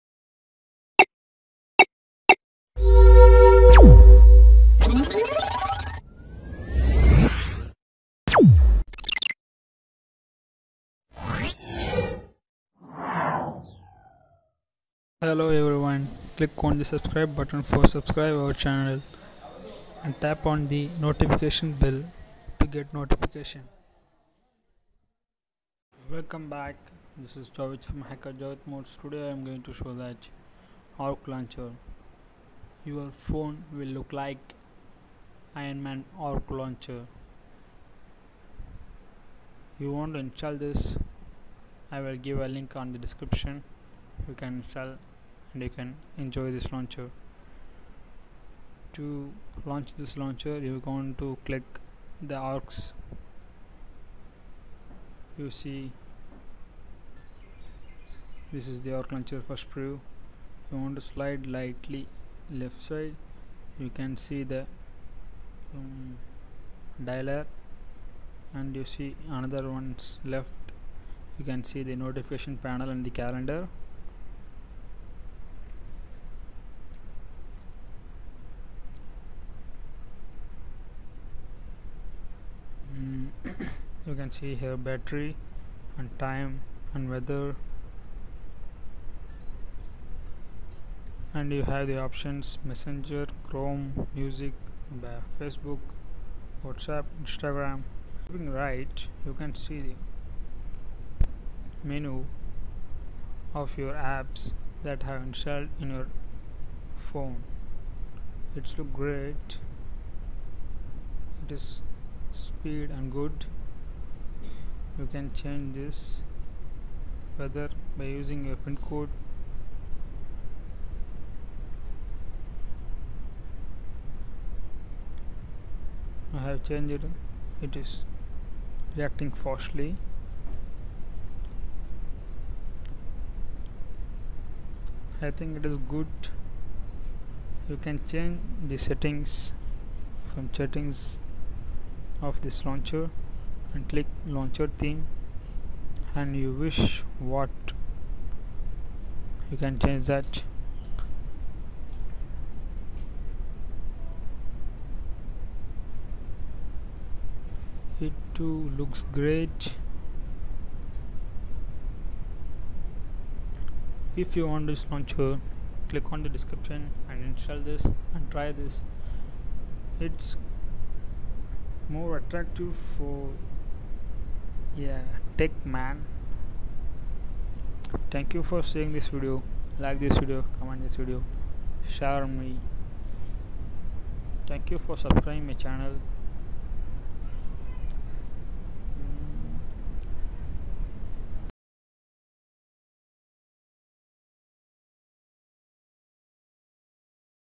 Change your smartphone into Jarvis systems (ARC LAUNCHER)- HACKERJAVIDMODS
Video:HJ

MY GEAR: LE2